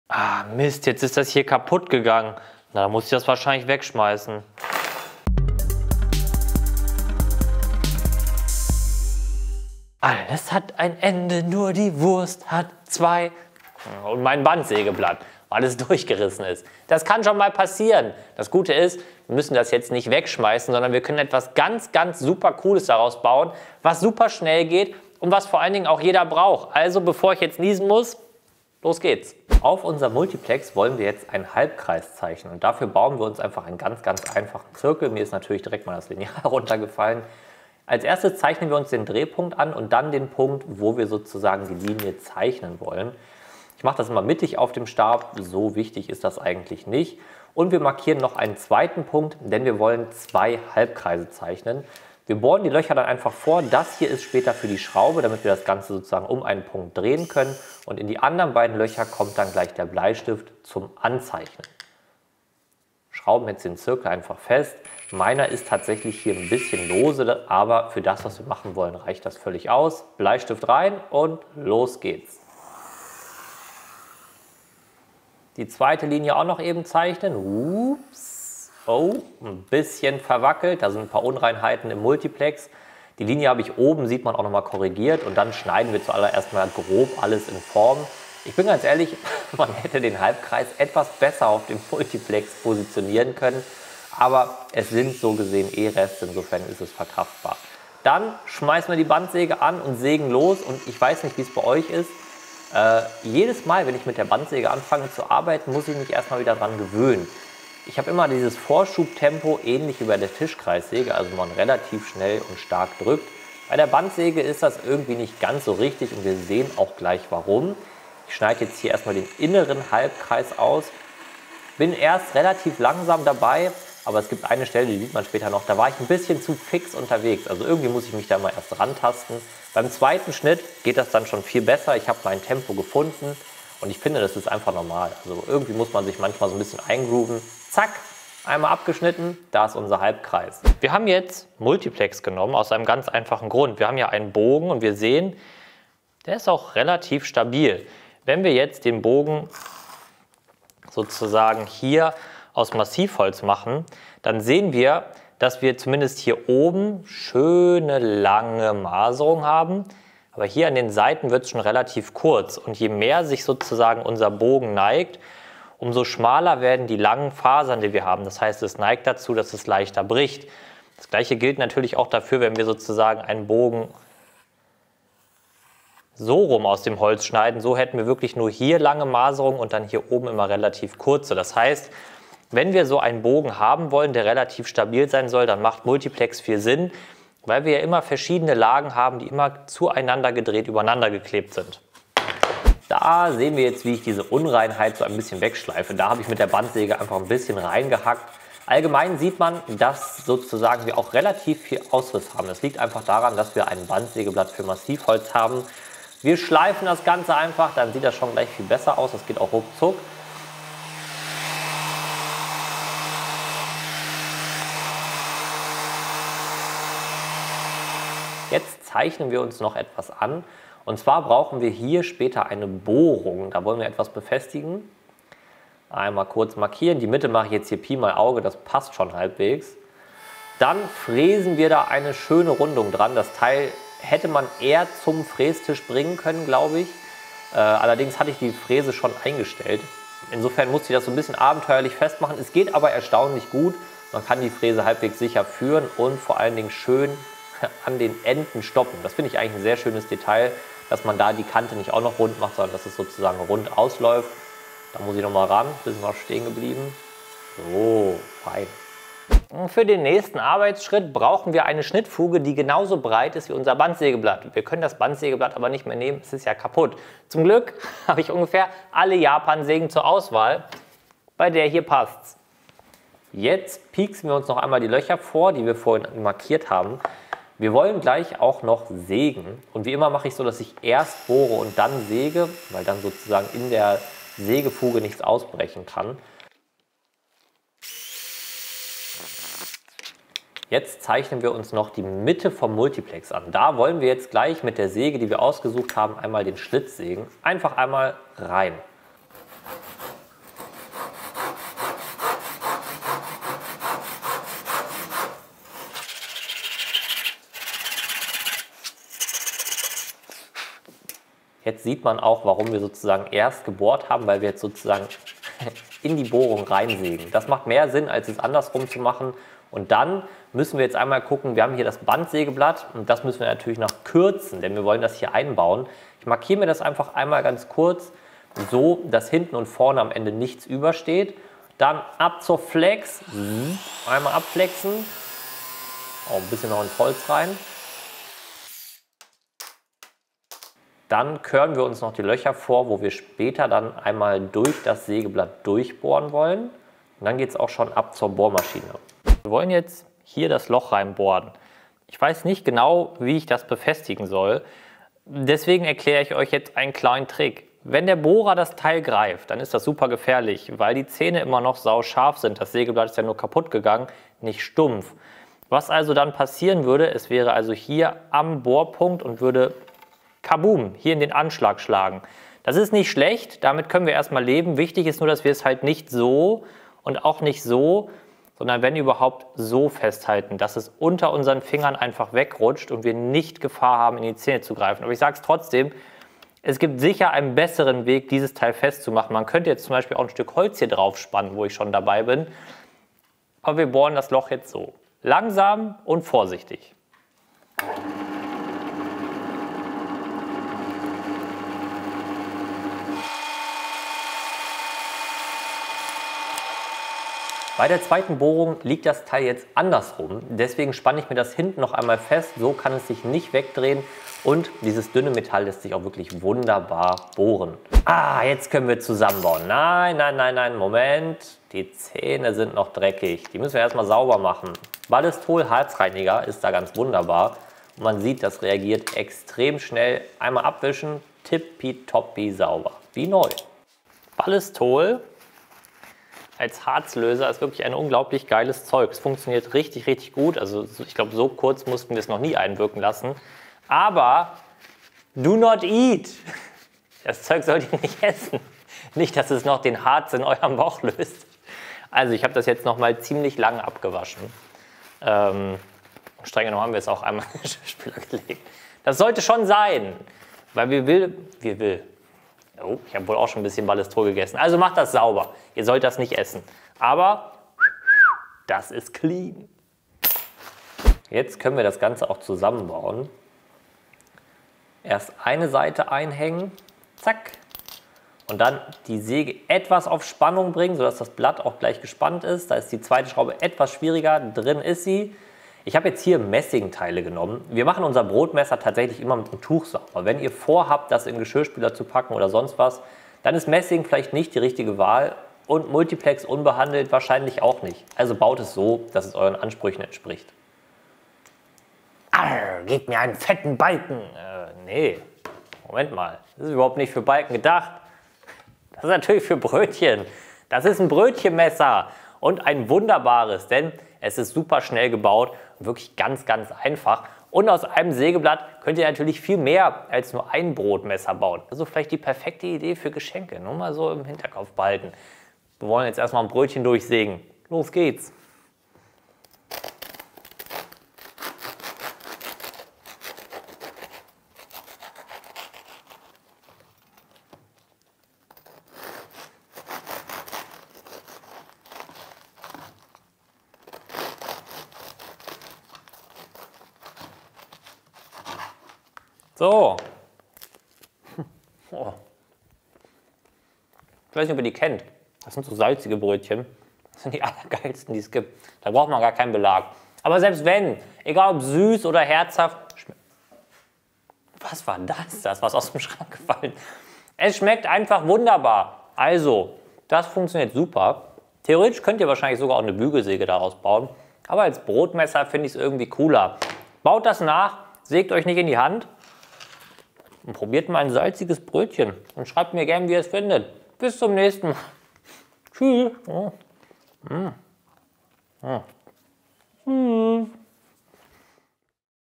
Darum solltest du dein KAPUTTES BANDSÄGEBLATT unbedingt BEHALTEN! | Jonas Winkler
Das Sägeblatt deiner Bandsäge ist kaputt gegangen - dann schmeiß es nicht weg! Ich zeige dir eine super einfache, schnelle und geniale Idee, was du aus dem Bandsägeblatt in deiner Werkstatt selber bauen kannst! Also, aufgepasst und nachgemacht!

► Lust auf Werkstatt-Vlogs? Dann hüpft mal rüber zu meinem YouTube Vlog-Zweitkanal @werktag.